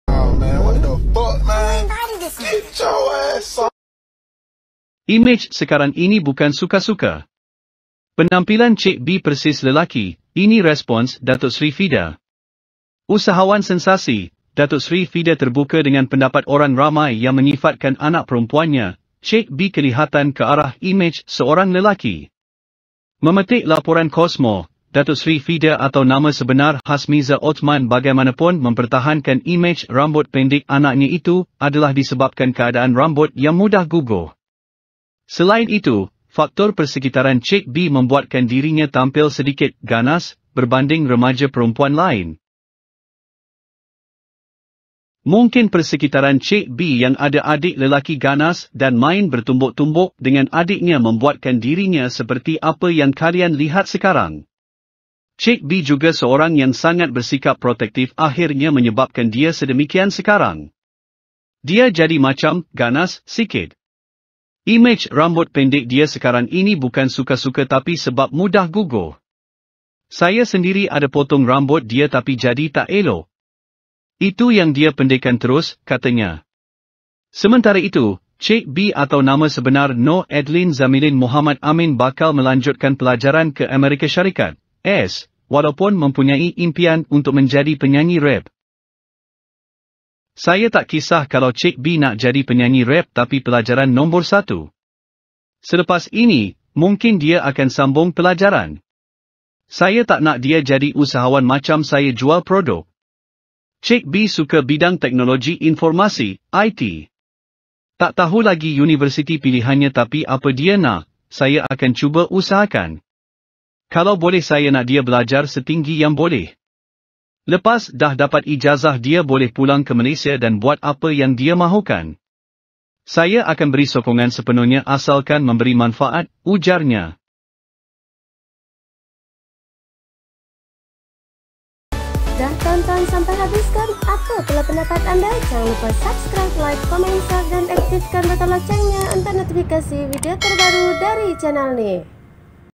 Penampilan Cik B Persis Lelaki, Ini Respons Datuk Seri Vida
Sumber : Xstra